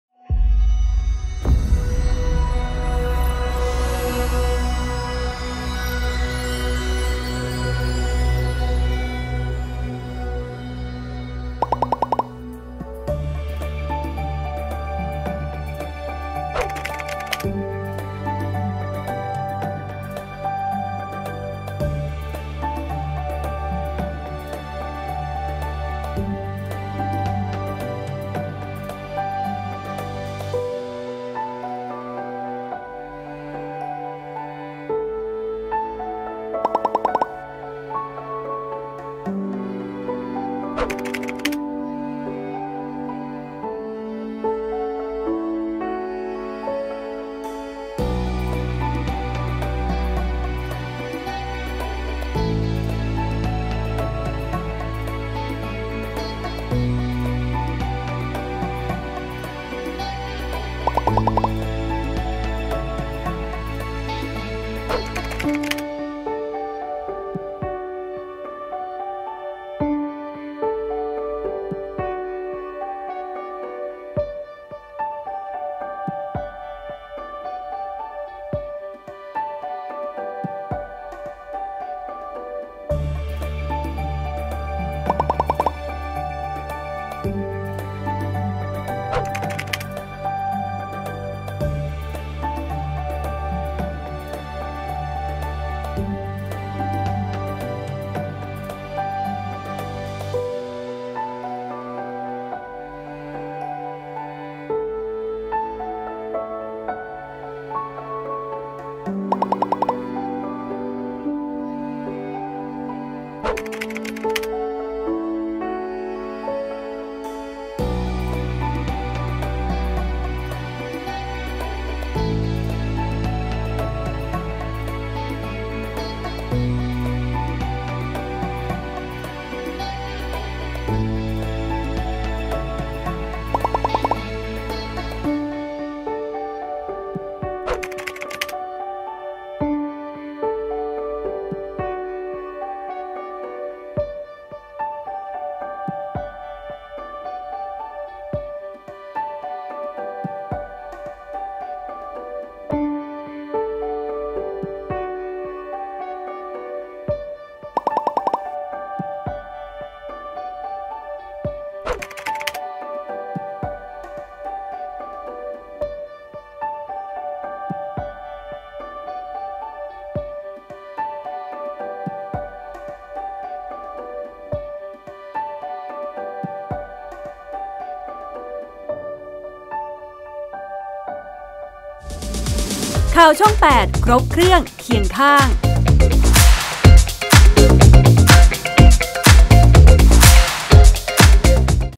ดวงประจำวันที่ 21 มกราคม 2566 | ข่าวช่อง8
ส่องดวงรายวัน ทั้ง 7 วัน การงาน การเงิน ความรัก รุ่งหรือร่วง รู้ก่อนใครที่นี่ เปิดชมเลย...

ขอบคุณข้อมูล : สมาคมโหรแห่งประเทศไทย ในพระบรมราชินูปถัมภ์